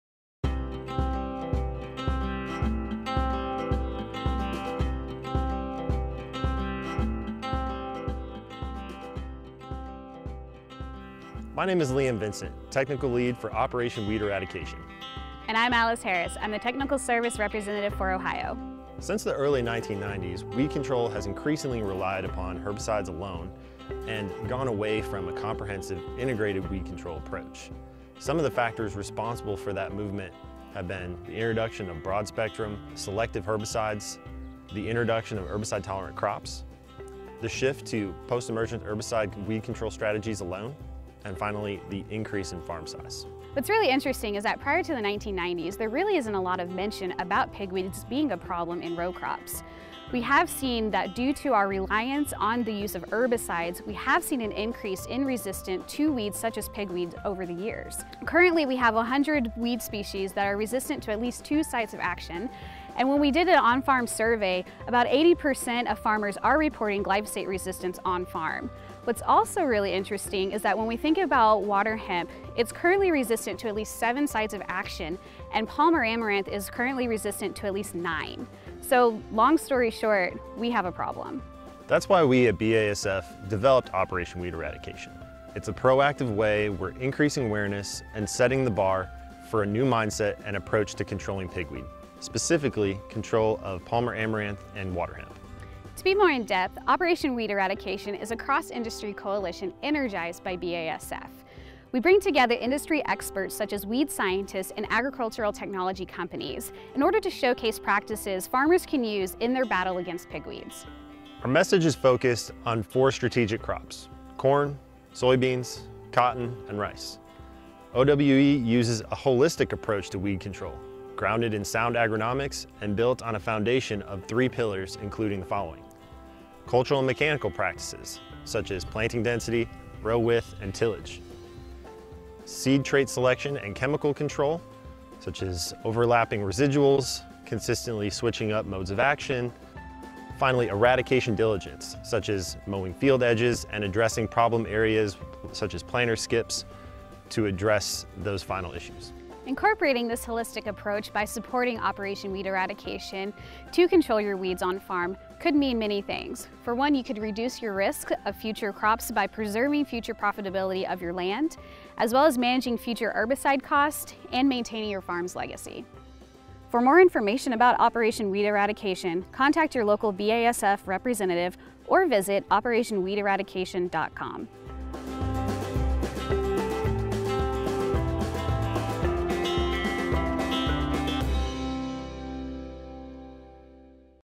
The Problem Weed - Pigweed Eradication 101
In episode 1 of Pigweed Eradication 101, BASF technical reps Liam Vincent and Alice Harris highlight the urgent issue of herbicide resistance in agriculture and introduces Operation Weed Eradication (OWE) practices as a response to pigweed. Learn how the reliance on herbicides alone since the 1990s has led to increased resistance in pigweeds, particularly Palmer amaranth and waterhemp. OWE brings together industry experts to showcase effective practices for controlling pigweed across key crops.

The OWE approach focuses on three pillars: cultural and mechanical practices, seed trait selection, and chemical control. It emphasizes optimizing planting density, row width, and tillage as well as using diverse seed traits and rotating different modes of action for chemical control with a focus on eliminating persistent pigweed hotspots.

Benefits of adopting a holistic approach to pigweed control include reduced risk of crop damage, preserved profitability of the land, managed herbicide costs, and maintaining your farm's legacy.